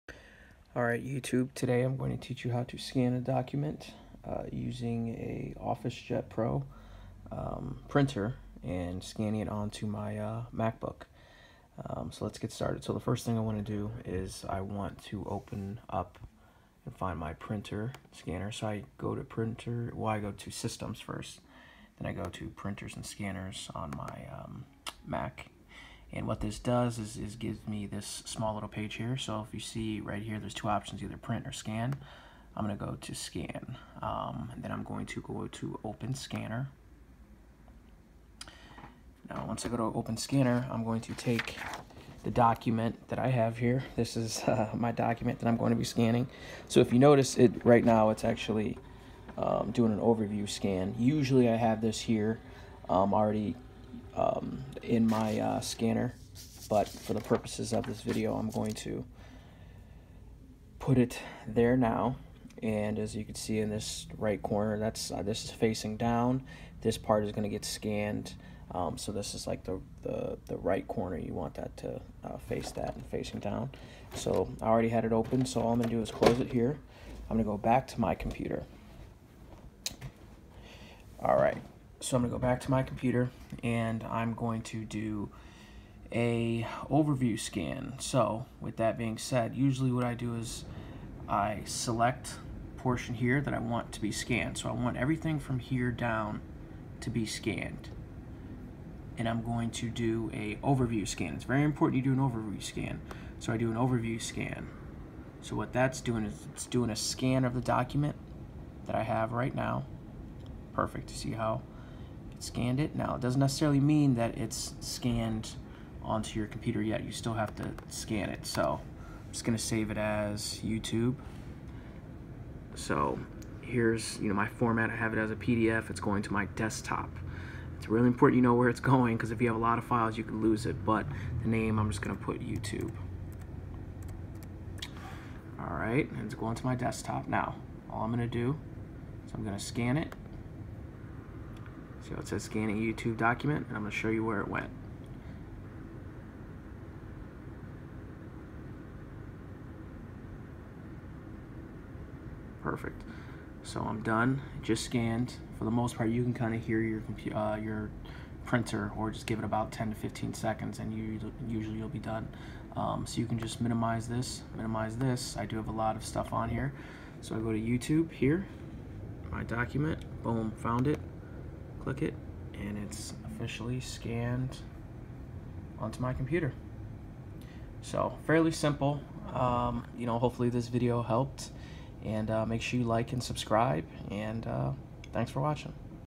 How to scan a document from a Officejet Pro printer to your computer
I teach you how to scan a document using a Officejet Pro printer